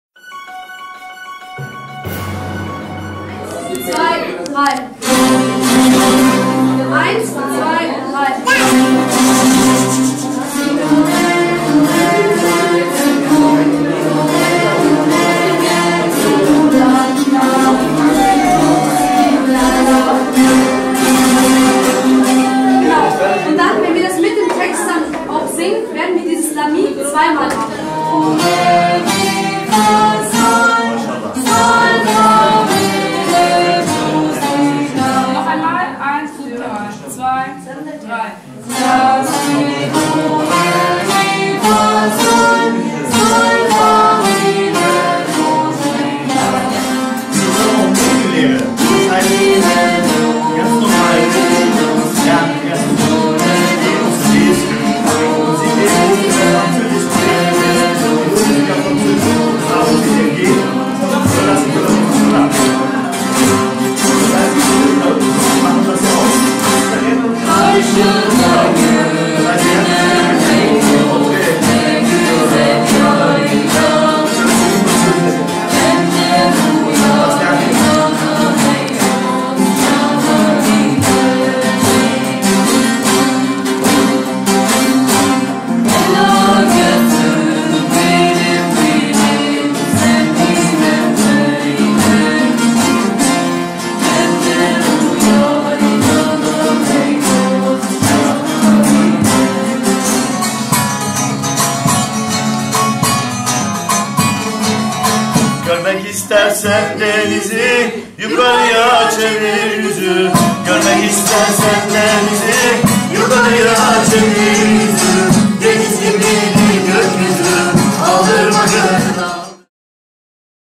Tag der offenen Tür bei den Aleviten
Tag der offenen Tür bei dem moslemisch-alevitischen Ehl i-Beyt-Verein in Schüttorf. Es wurden die Jugendarbeit, aber vor allem die musikalische Ausbildung an der Saz präsentiert.